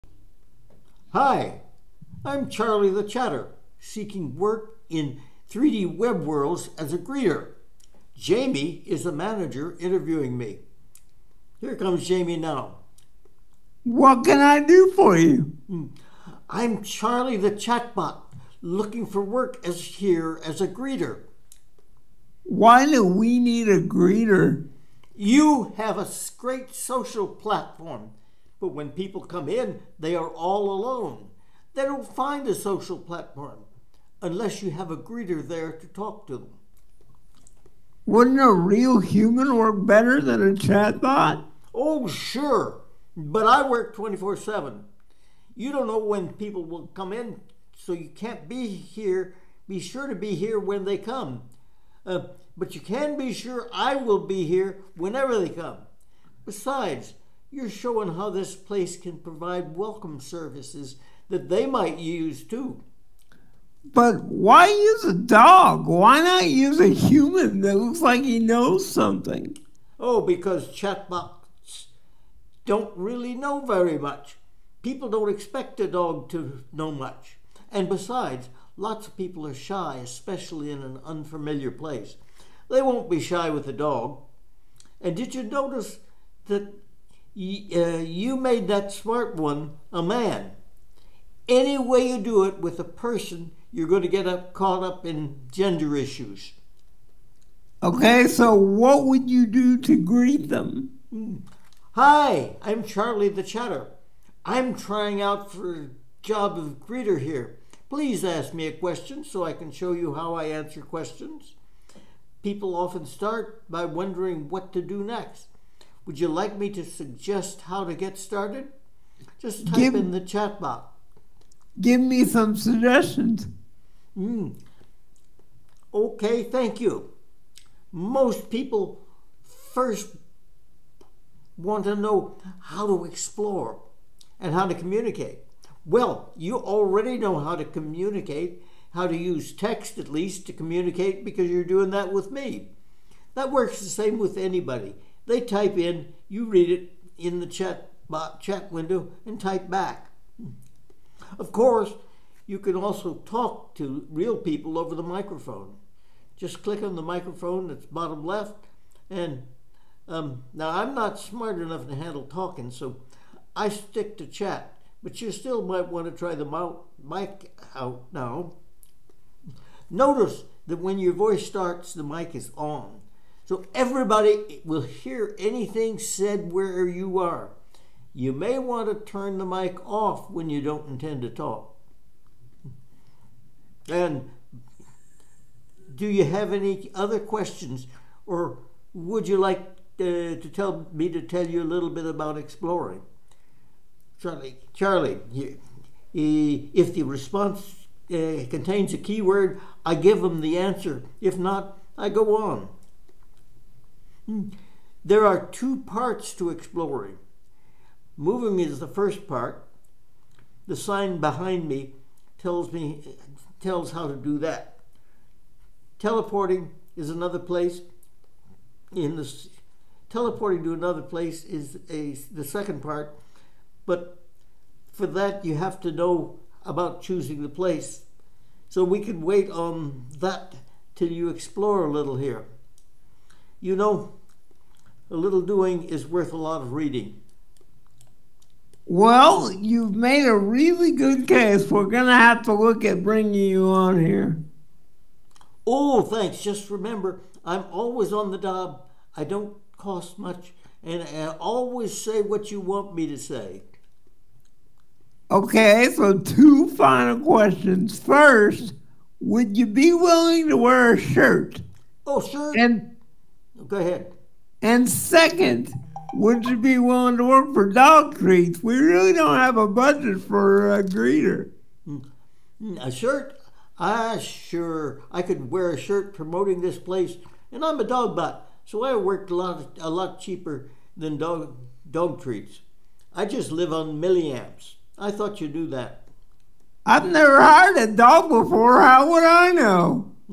Charlie wants work as a greeter bot.
Works 24/7, always says what you wrote,
works for milliamps, makes a virtual world social.
Enter as guest, no need to register.